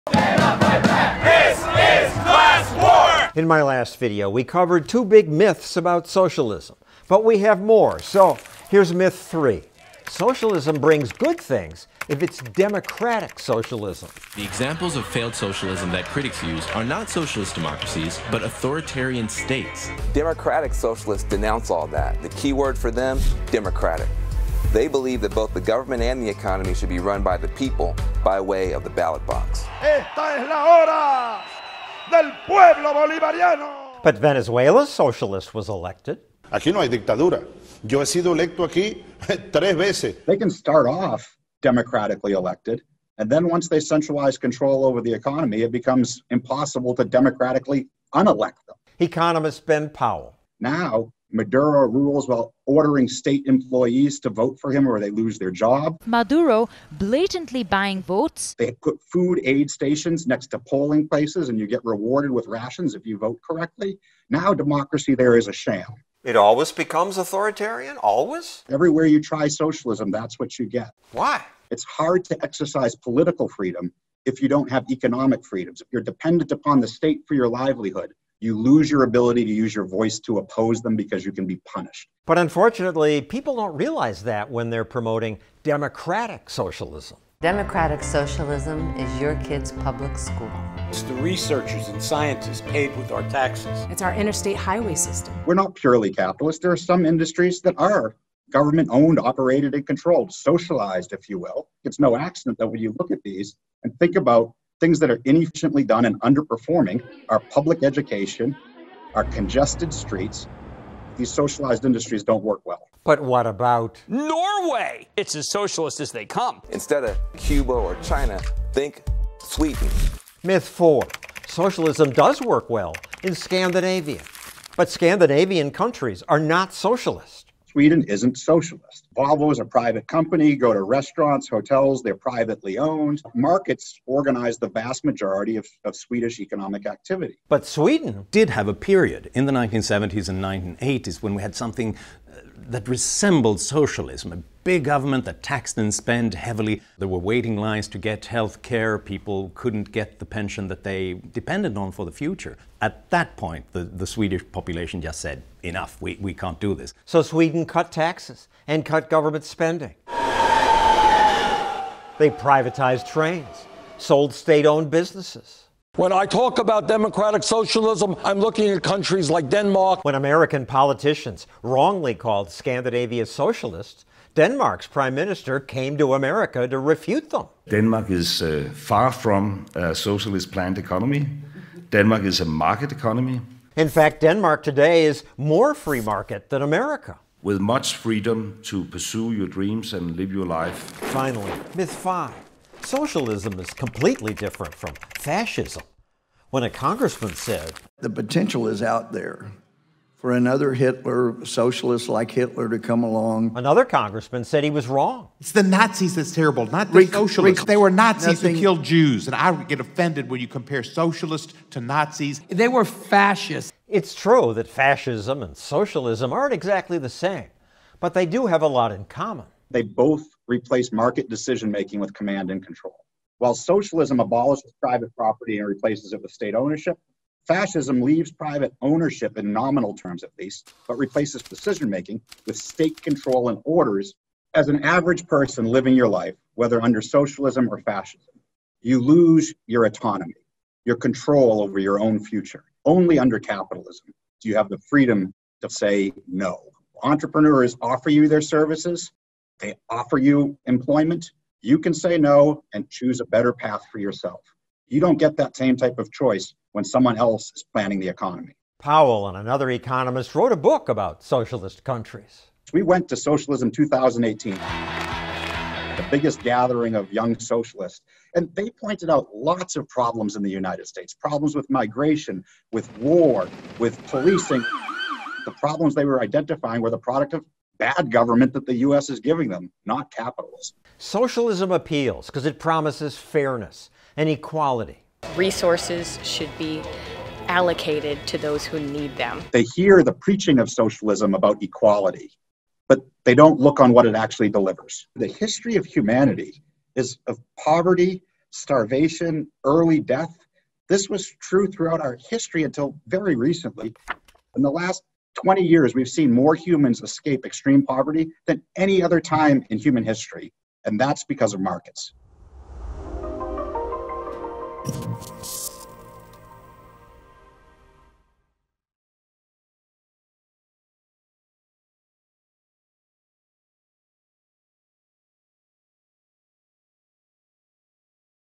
5 Socialism Myths: Part 2
Many young people love socialism. They don’t like capitalism. They don’t notice what capitalism give them. They protest in Nikes, and tweet about it from their iPhones.

In this video, I continue where I left off in my last video, and debunk more myths about socialism.

Don't miss the weekly video from Stossel TV.
Myth #3: Socialism brings good things if it’s DEMOCRATIC socialism.

“Democratic socialists believe both the government and the economy should be run by the people by way of the ballot box,” says Van Jones approvingly on CNN.

By the ballot box—that’s how Venezuelan dictator Hugo Chavez took power.

“They can start off democratically elected [but] once they centralize control over the economy, it becomes impossible to democratically unelect them,” explains economist Benjamin Powell.  “Now democracy in Venezuela is a sham.”

Socialism ALWAYS ends up authoritarian.  We explain why in the video above.